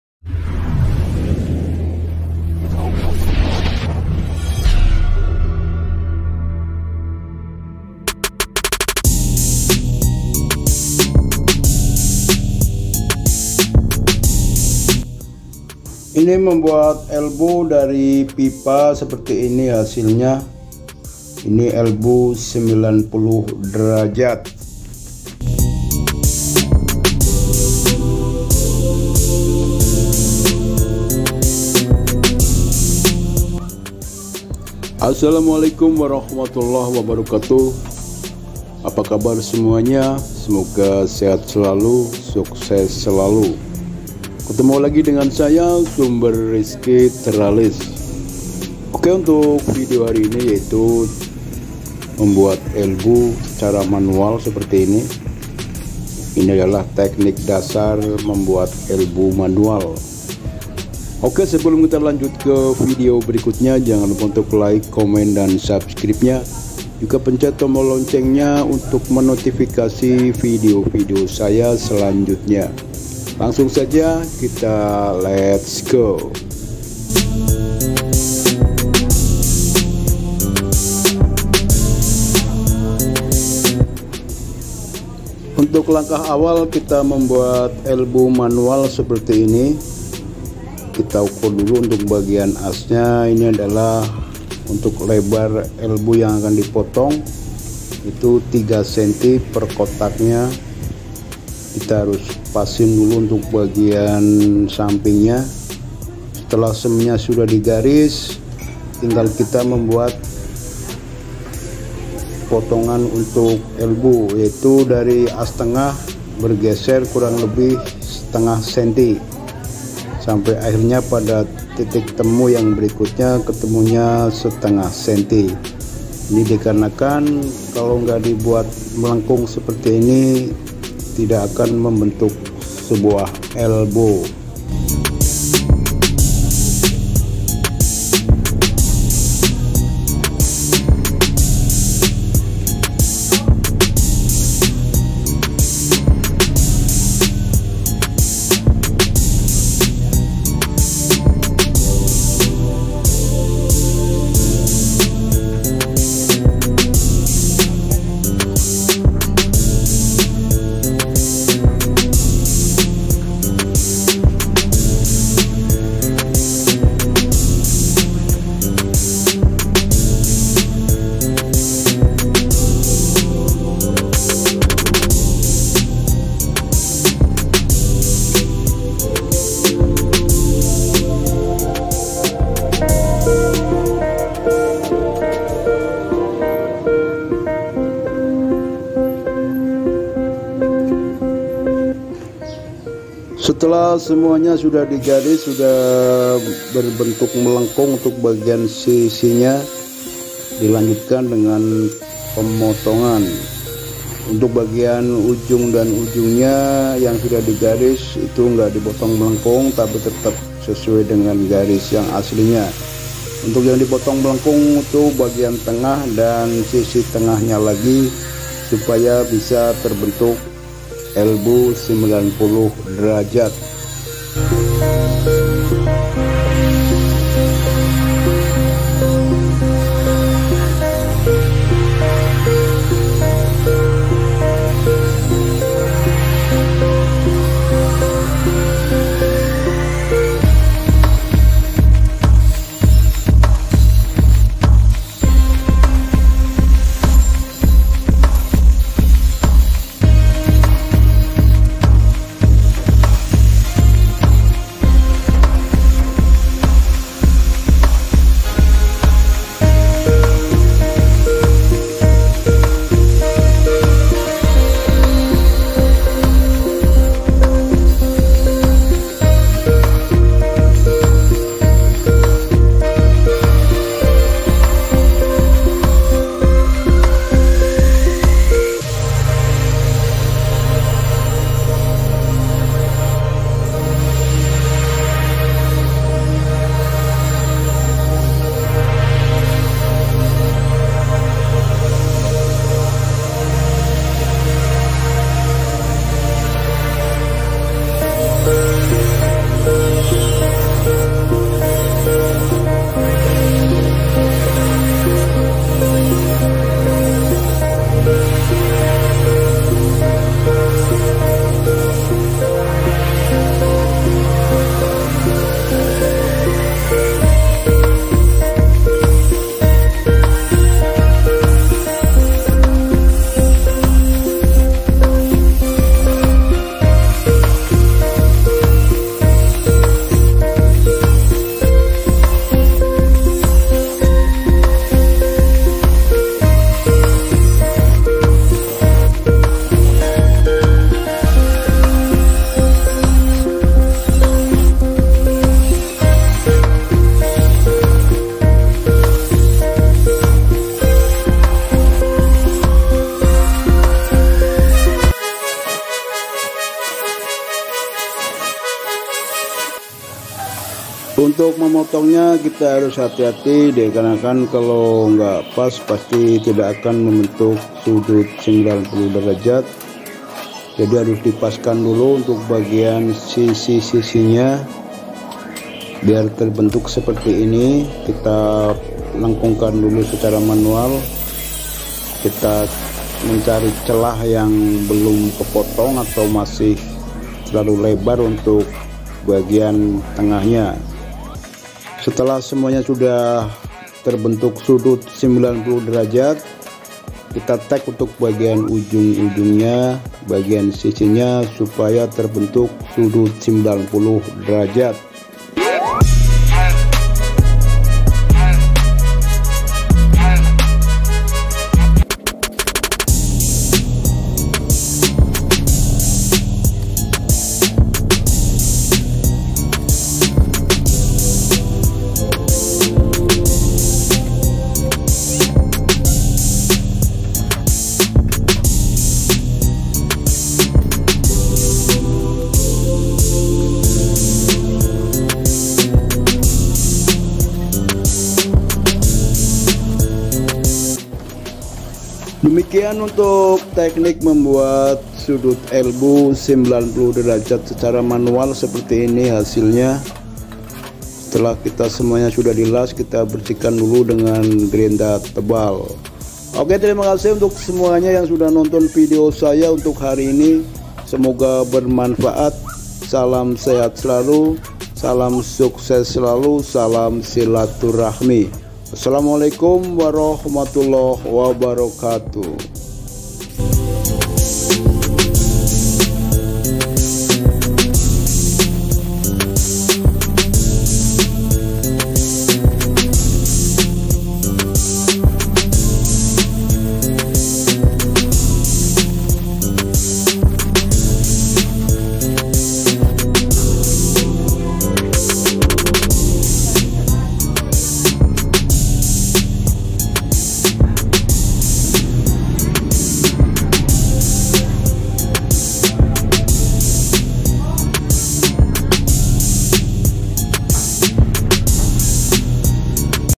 cara pengelasan|| pengelasan dan cara membuat elbow manual || how to make elbow manually
asalamualaikum sahabat semuanya salam sehat selalu,salam sukses selalu,salam silaturahmi,untuk video hari ini adalah cara pengelasan membuat elbow manual,cara ini sangat simpel dan praktis.tonton terus videonya semoga bermanfaat.